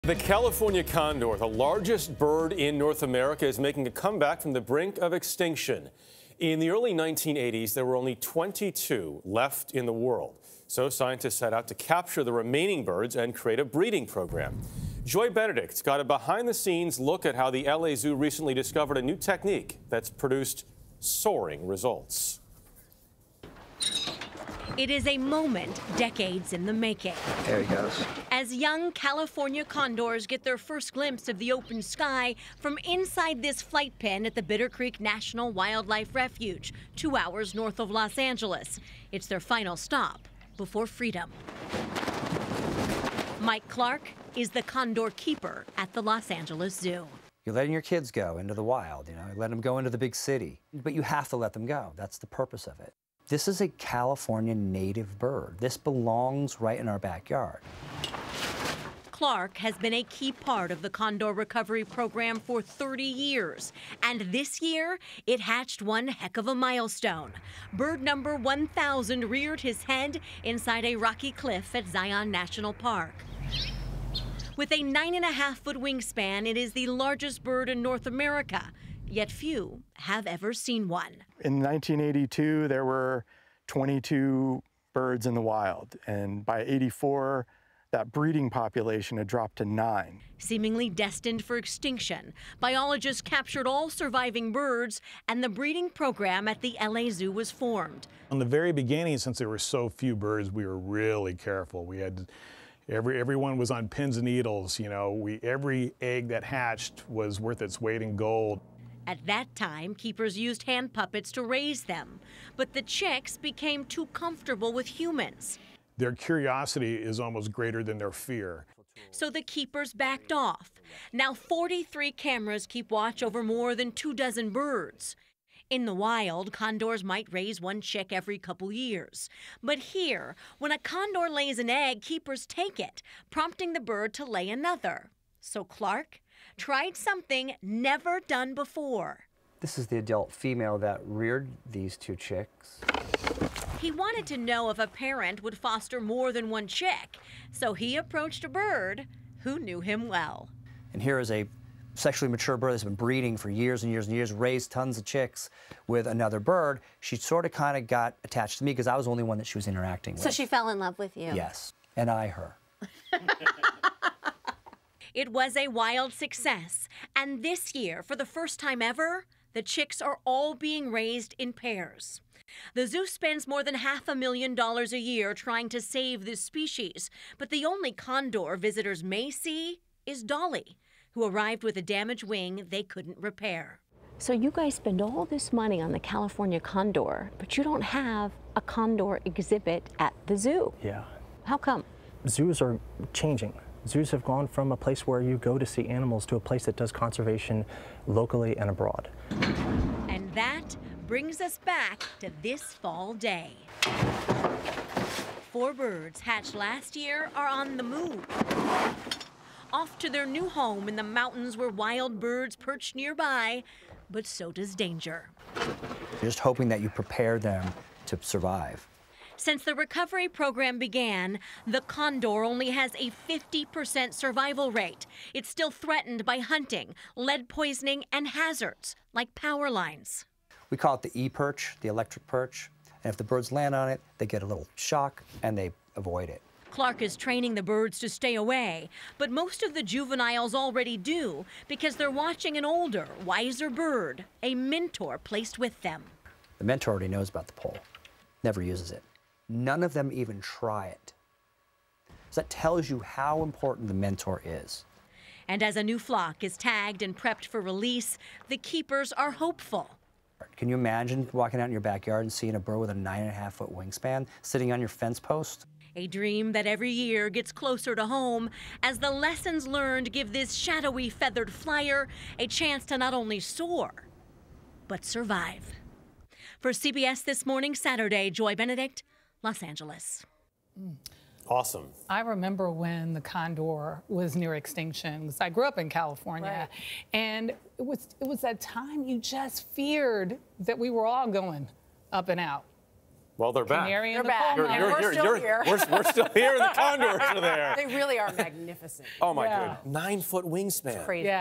LA Zoo uses new tactic to boost California condor population
The California condor, the largest bird in North America, is making a comeback from the brink of extinction. In the early 1980s, there were only 22 left in the world. So scientists set out to capture the remaining birds and create a breeding program. Joy Benedict got a behind-the-scenes look at how the Los Angeles Zoo recently discovered a new technique that's produced soaring results.